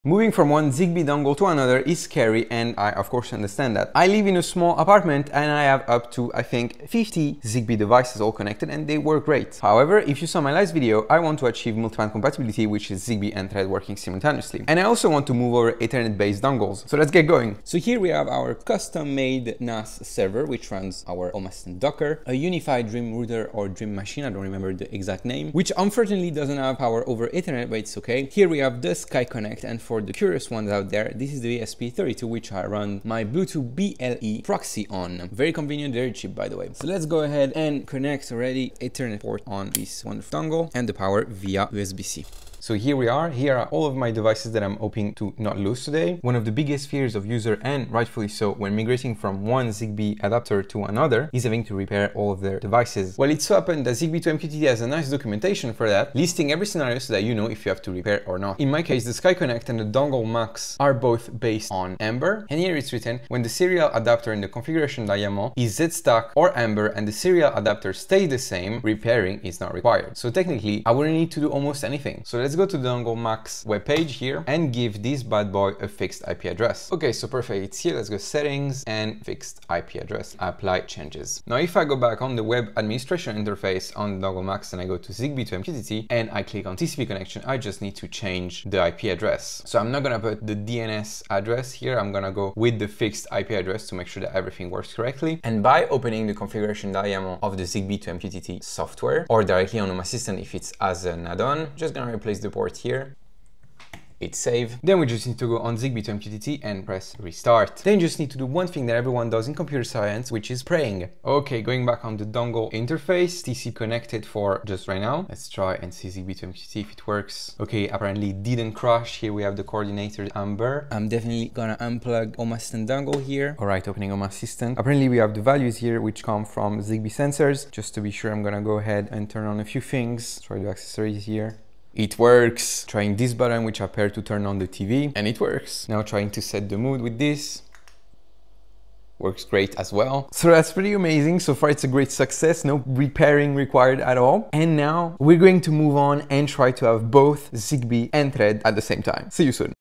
How to migrate from one ZigBee dongle to another
Following up on my last post talking about Matter, ZigBee and Thread, this is my attempt at doing multi-pan by switching from the Home Assistant SkyConnect (I don't remember the new name), to a Dongle Max by Sonoff. The first step is making sure we don't lose the existing ZigBee network!